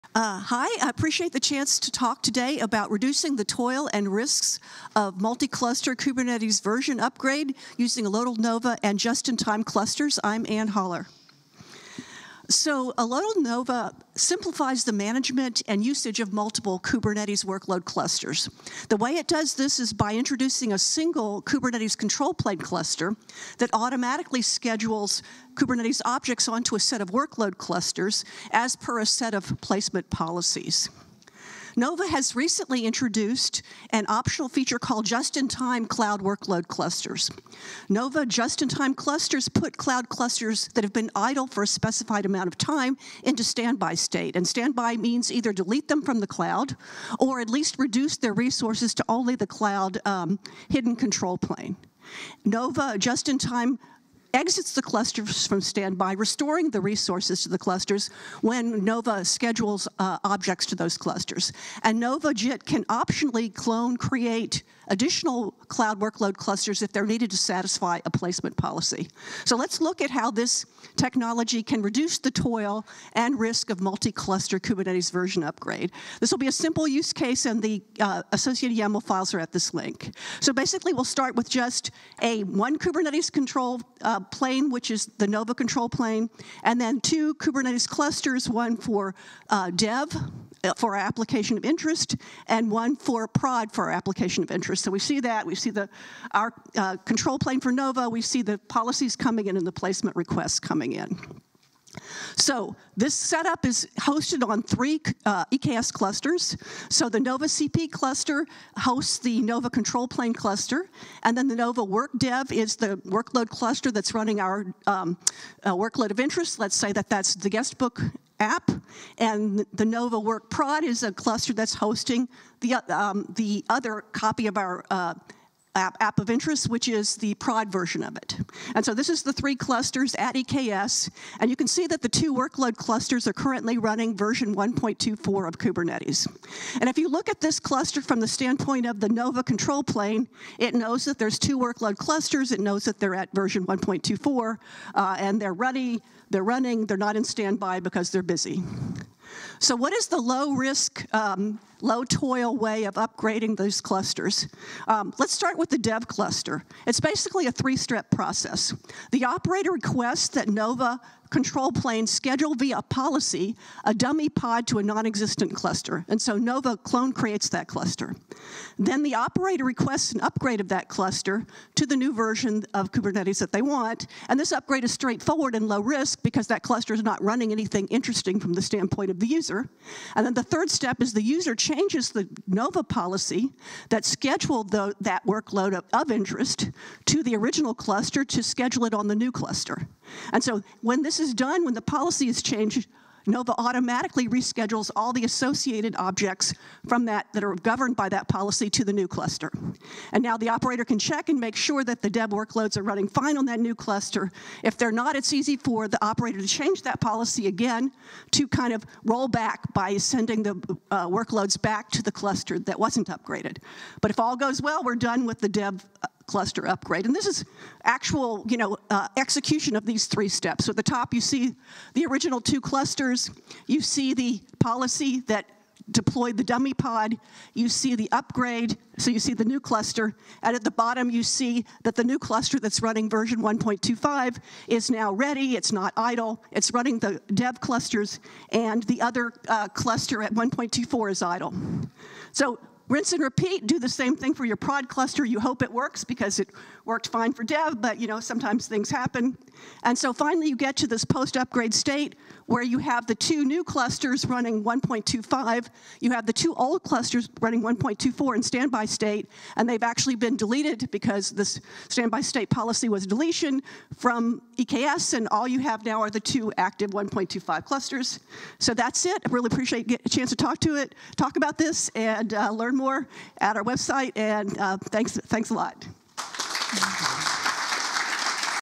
Reducing the Toil and Risk of Multi-cluster K8s Version Upgrade using Elotl Nova + JIT Clusters
Lightning Talk by Anne Holler, Elotl

Cloud Native Rejekts EU 2023